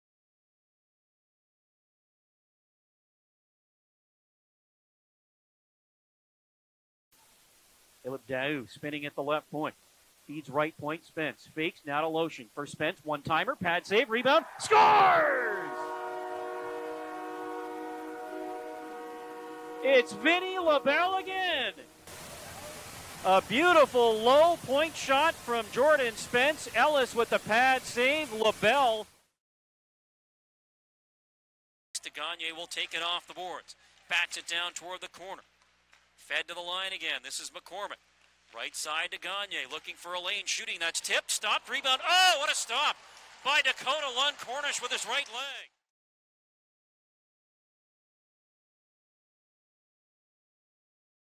Dakota Lund-Cornish's toe save vs. "Vinny Labelle again!"
Dakota Lund-Cornish stretches to make a big save with his toe, while Vincent Labelle's second goal of his debut electrified the building!

Vote for your favourite — Make your voice heard in the comments below! You have 24 hours to vote

For more details, and to stay up to date on the bracket,